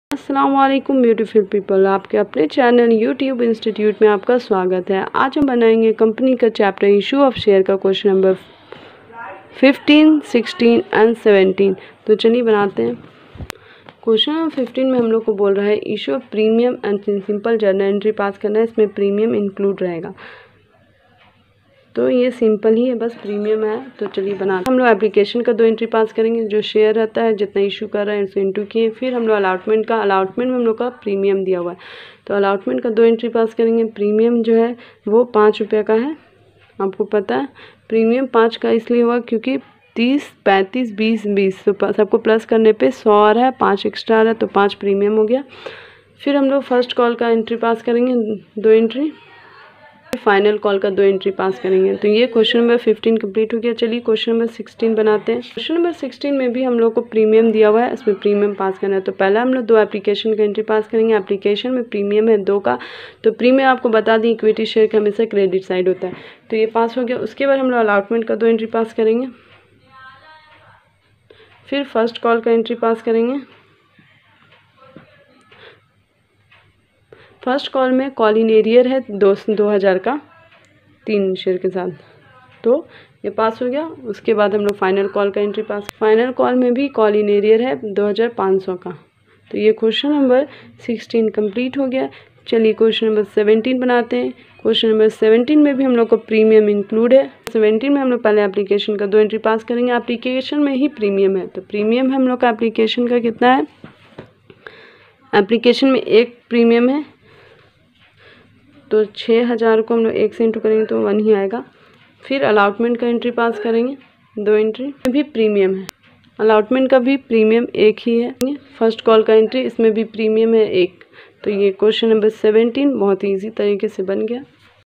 Questions no 15,16,17#COMPANY#(ISSUE OF SHARE)FOR CLASS 12#ACCOUNT#S.K SINGH BOOK-YOUTUBE INSTITUTE
In this video questions no 15,16,17 company#(issue of share) for class 12 account# s.k Singh book and each questions are details in explain